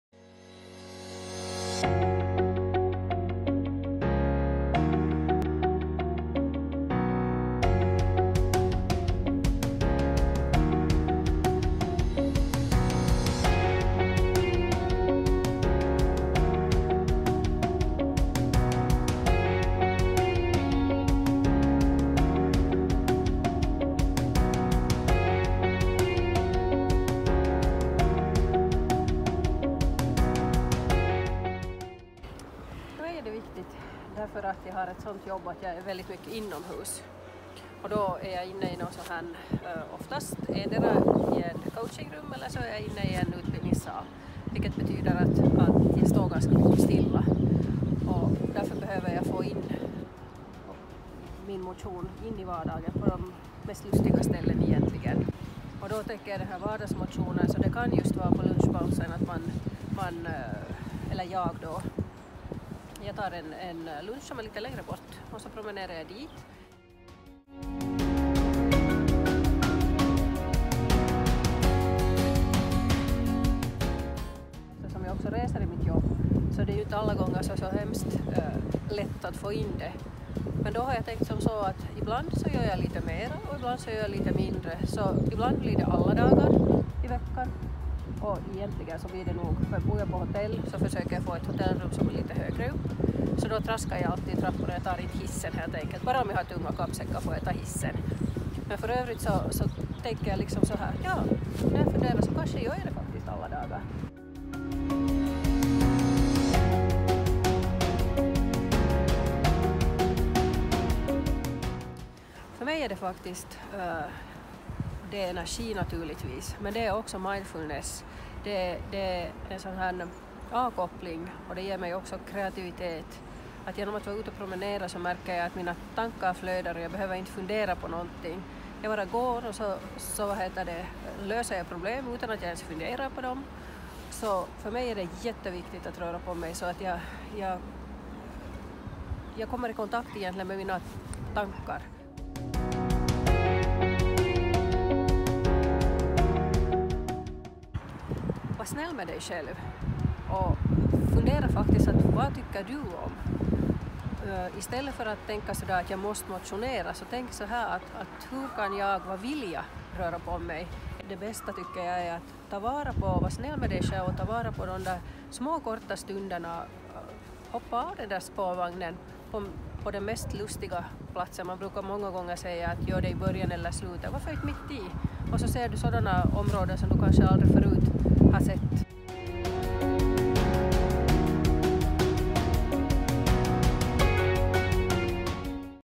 Månadens motionär - Christine Suvanto
Varje månad presenterar vi månadens motionär och olika motionsformer. Den här månaden får ni träffa företagare, styrelseledamoten och motionären Christine Suvanto som berättar hur hon helst rör på sig och hur ofta.